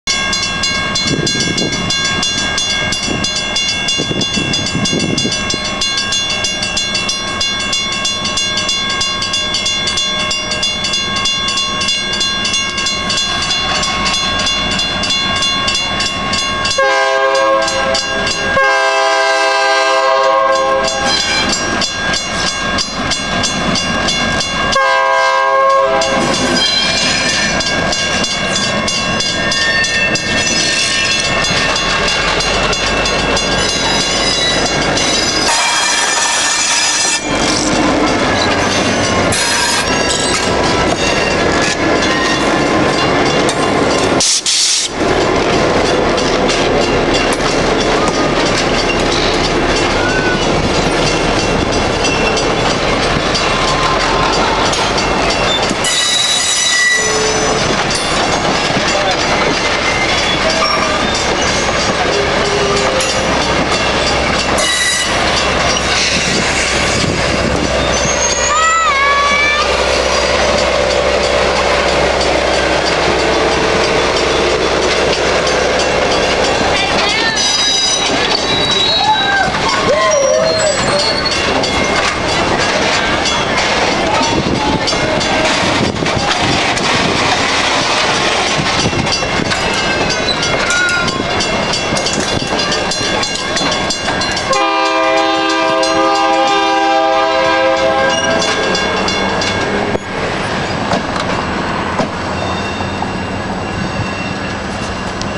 GVRR 2199 ENTERING STOCKYARDS STATION
CROSSING 23RD ST IS GVRR 2199 AS IT ENTERS STOCKYARDS STATION.